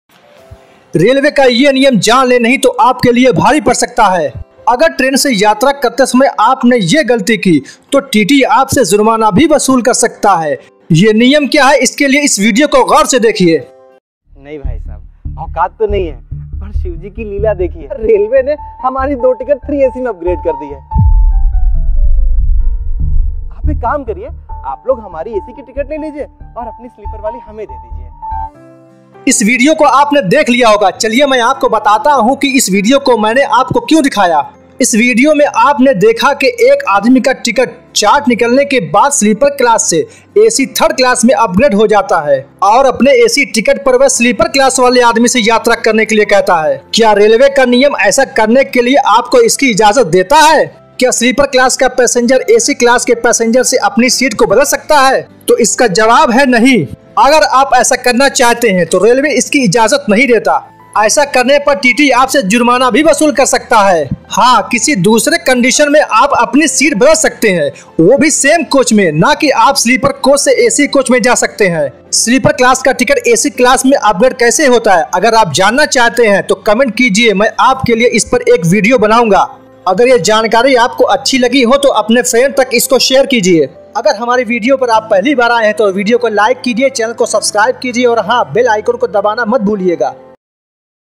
indian railway rules 2024 | railway waiting rules | waiting se ja sakte hai ya nahi | railway news 🚈
waiting ticket se Yatra kar sakte hai ya nahi